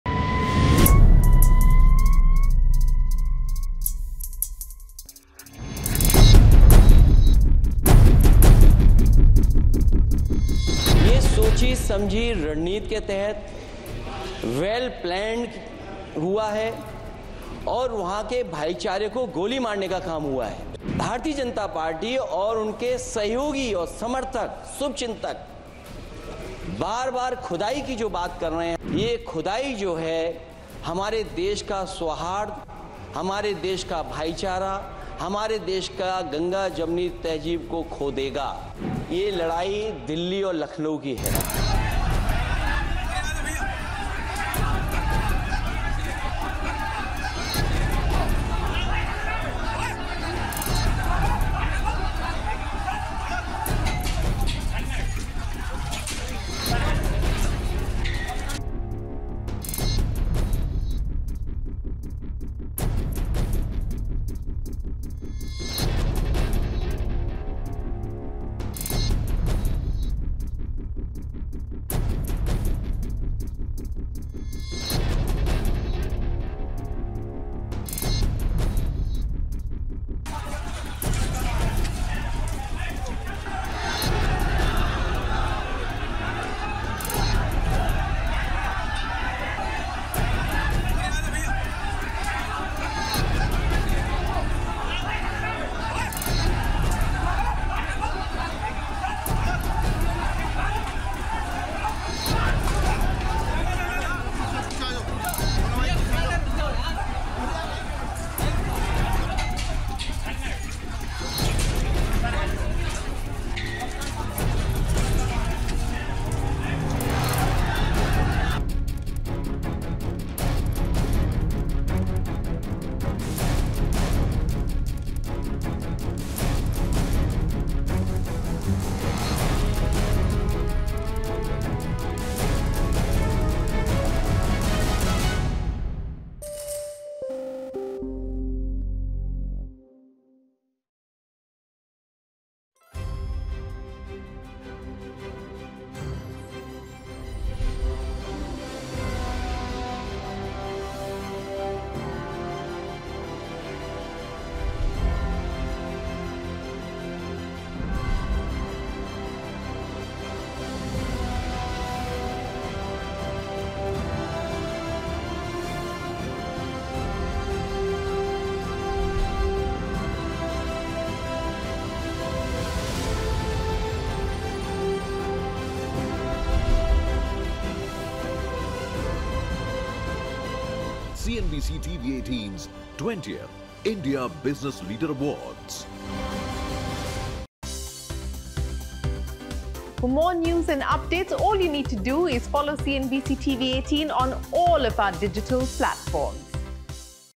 'Those Who Want To Dig Everywhere...': Akhilesh Yadav In Parliament On Sambhal Violence | N18V
'Those who want to dig everywhere will, one day, lose the brotherhood of the country'. said Samajwadi Party MP Akhilesh Yadav in Parliament on the Sambhal violence. He further alleged that the incident was part of a 'well-planned conspiracy'.

n18oc_business